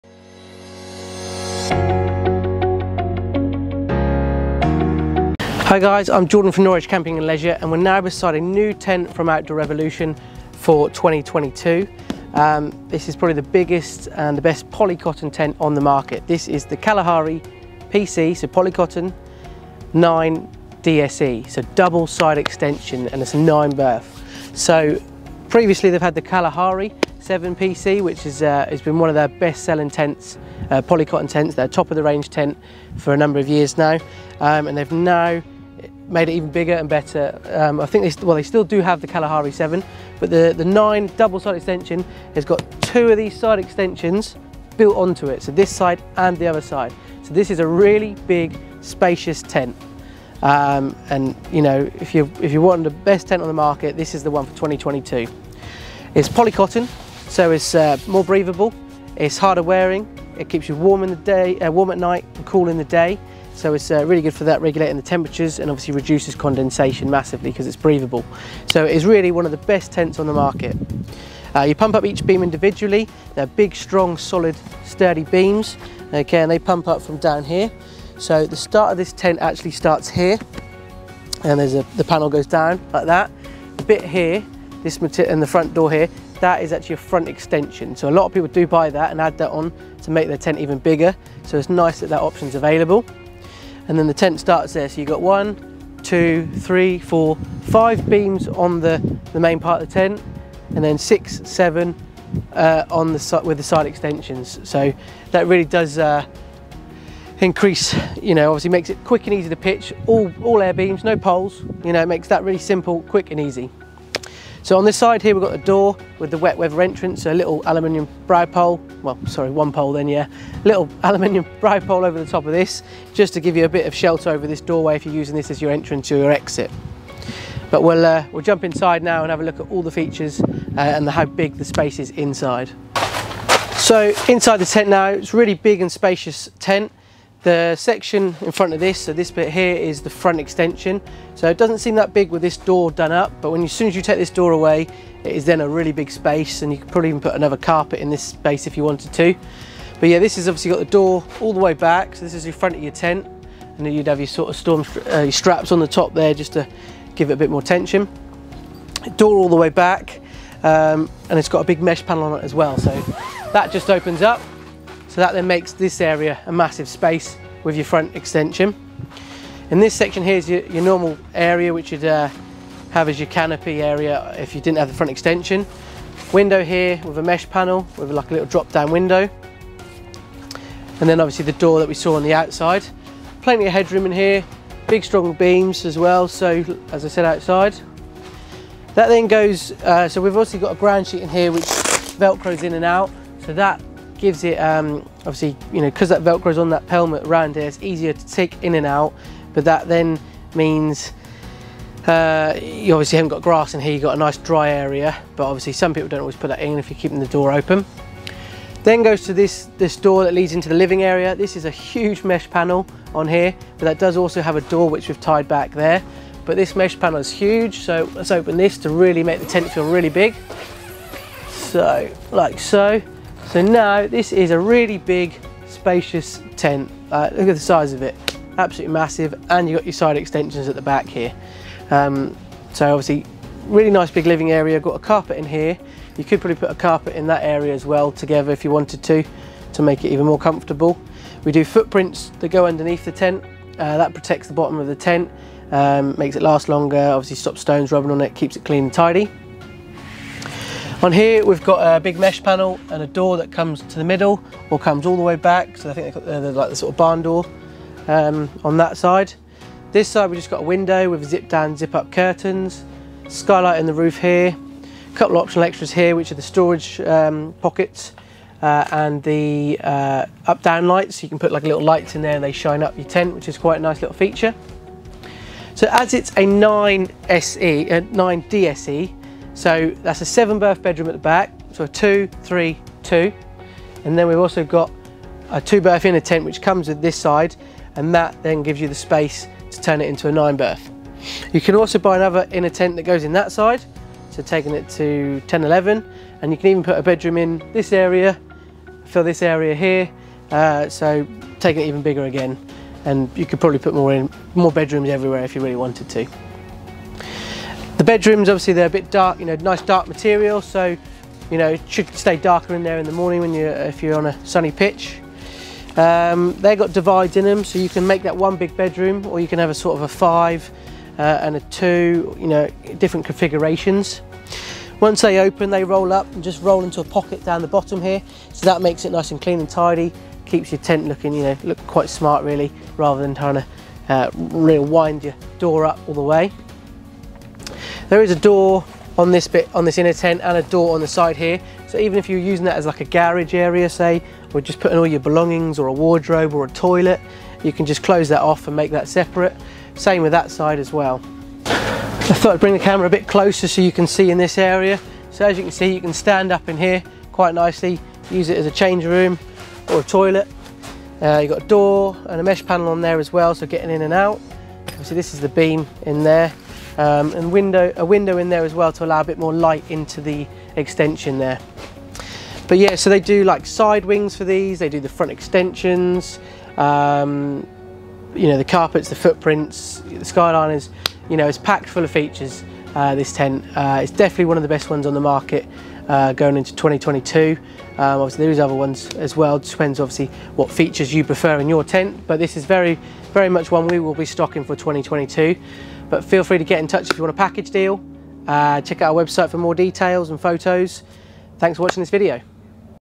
Is this the BEST Polycotton Air Tent | Outdoor Revolution Kalahari 9DSE 2022
The new Outdoor Revolution Kalahari 9DSE is the largest polycotton air tent in the market for 2022, with the double side extension built on you can really maximize space.

Sleeps 9 but can go up to 11 with the extra inner tents
Lifetime Guarantee on the air tube when registered
Package deals available